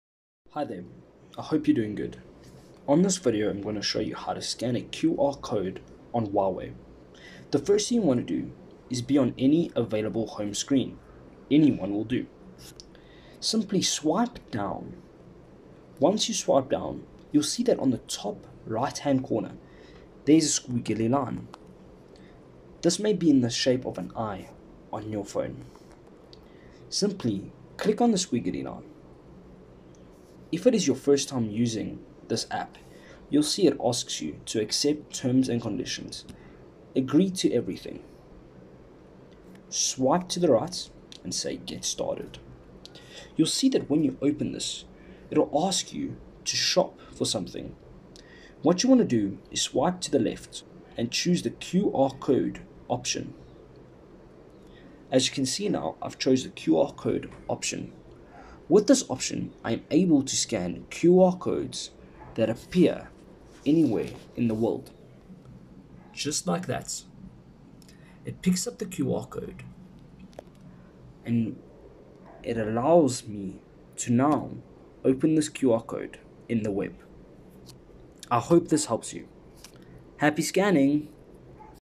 Easiest way to scan a QR Code on HUAWEI!
The Steps

1)Be on any home screen

2)Swipe down

3)Click on the eye looking icon on the top right.

4)Accept all T&C's

5)Swipe to the left and choose the QR code option.

6)Scan the QR Code

Happy Scanning!